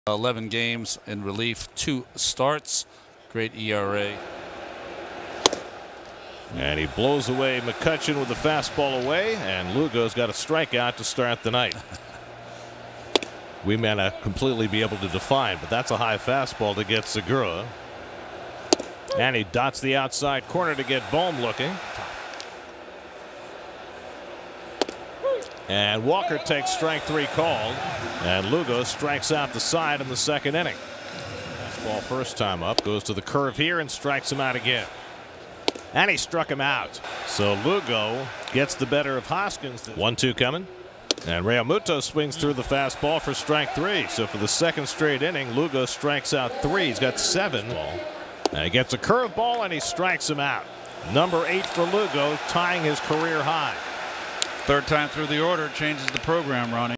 Lugo Ties Career High for Strikeouts
9/5/20: Seth Lugo ties his career high with 8 strikeouts over 5 innings as the Mets defeated the Philadelphia Phillies 5-1.

Category
Sports